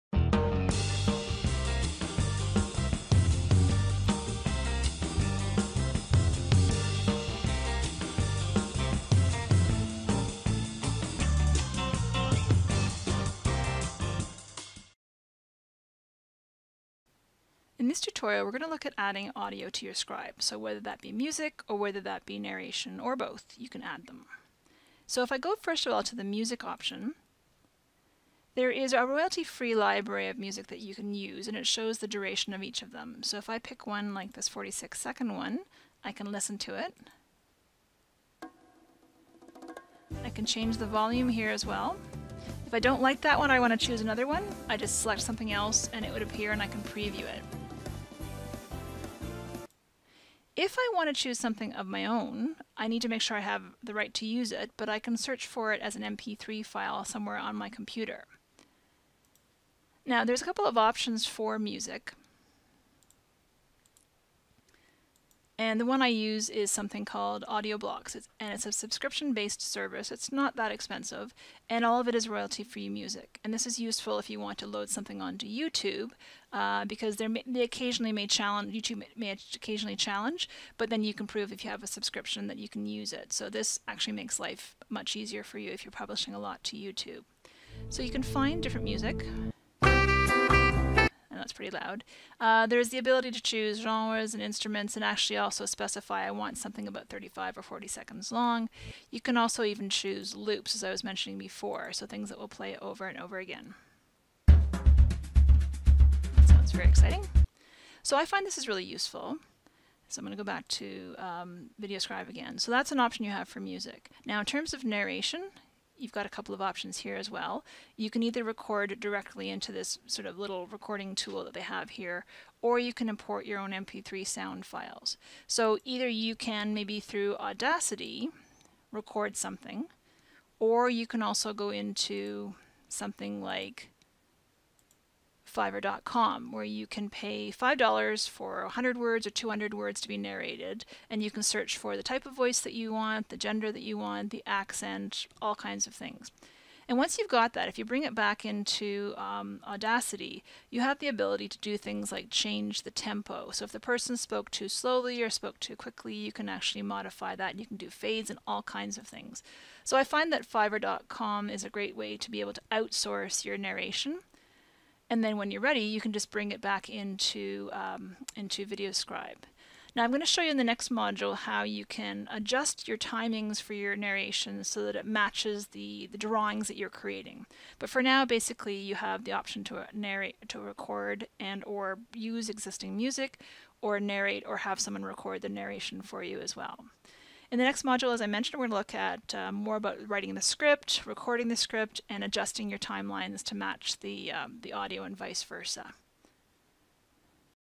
VideoScribe Tutorial 3 : Adding Audio
Adding music and narration using free and subscription-based services.